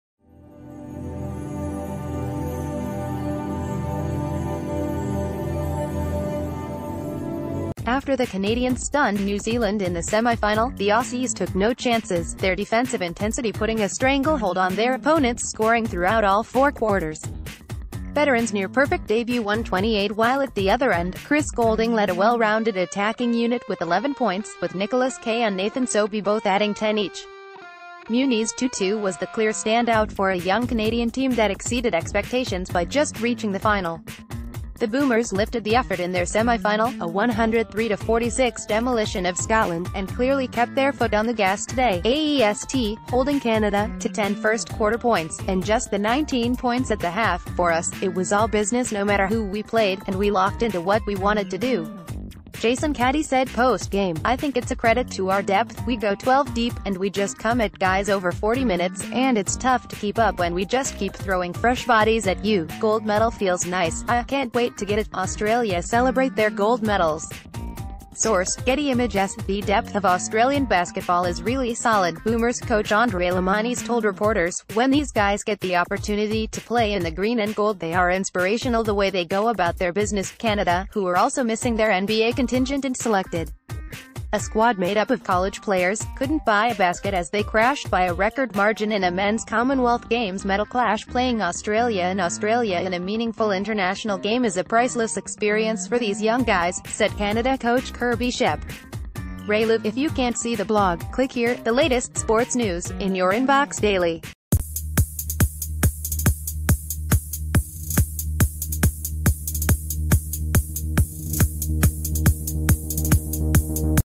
Breaking News | Comm Games Basketball Gold Medal Game: Australian Boomers vs Canada, scores, updates
After the Canadians stunned New Zealand in the semi-final, the Aussies took no chances; their defensive intensity putting a stranglehold on their opponents’ scoring throughout all four quarters. Veteran's near perfect debut1:28While at the other end, Chris Goulding led a well-rounded attacking unit with 11 points, with Nicholas Kay and Nathan Sobey both adding 10 each. Munis Tutu was the clear standout for a young Canadian team that exceeded expectations by just reaching the final. The Boomers l...